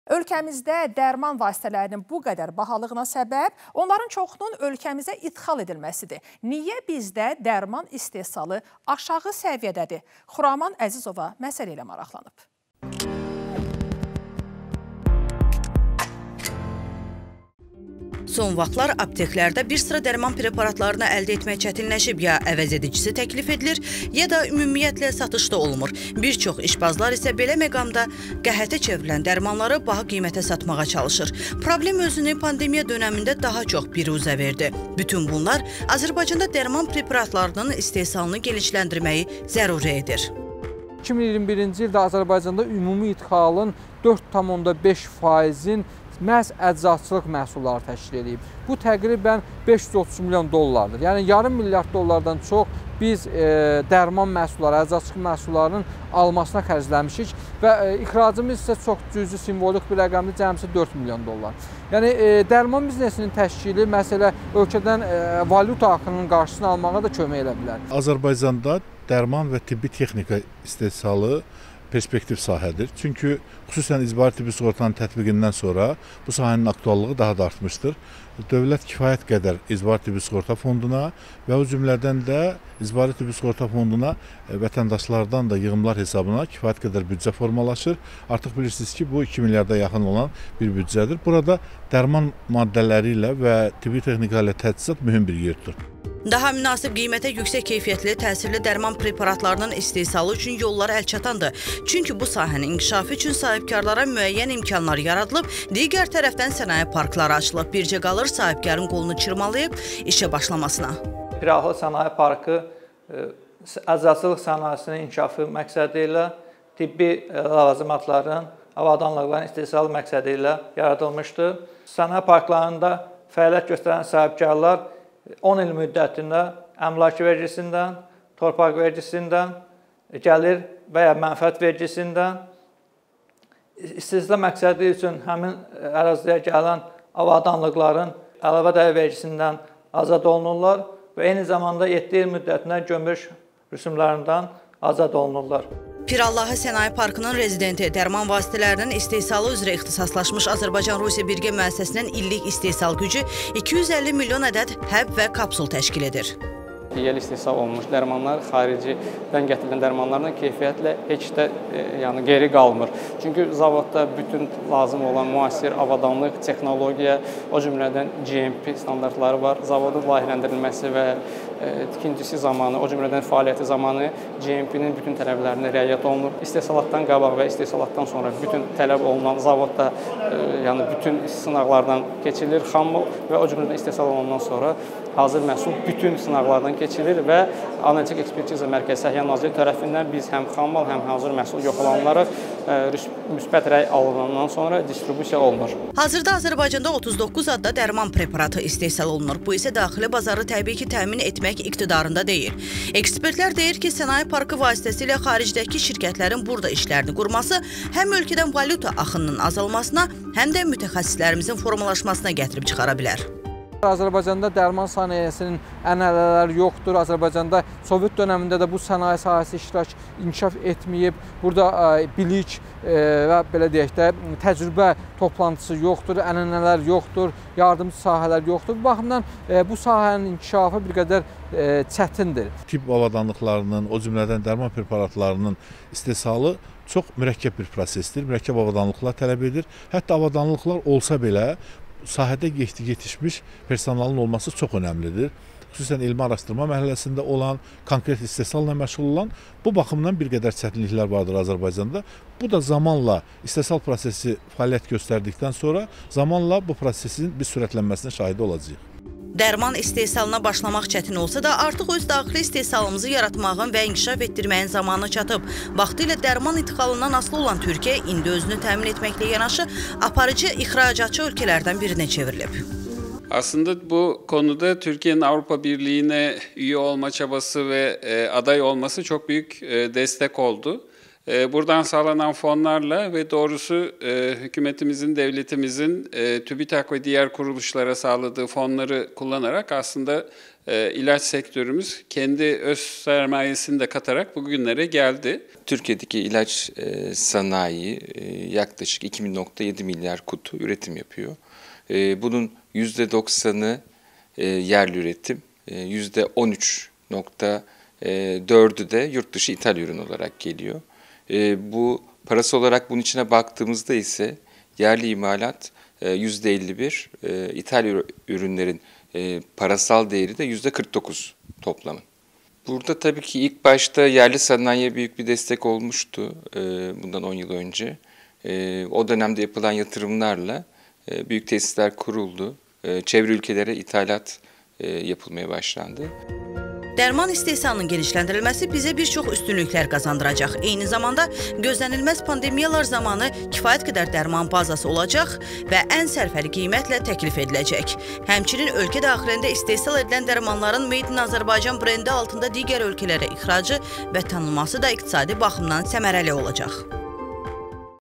Dərmanlar niyə bahadır?
Ölkəmizdə dərman vasitələrinin bu qədər bahalığına səbəb həmin dərmanların ölkəmizə idxal edilməsidir. Spirt, pambıq və maskanı çıxsaq. Pandemiya olmasaydı, yəqin ki, maskanı da hələ Çindən alırdıq. Bir sözlə, çox az sayda dərman vasitəsi ölkəmizdə istehsal olunur, amma əhalinin istifadə etdiyi əsas həblərin əksəriyyəti idxal edilir. Niyə bizdə dərman istehsal edən müəssisə və zavodlar çox azdır? Xuraman Əzizova məsələ ilə maraqlanıb.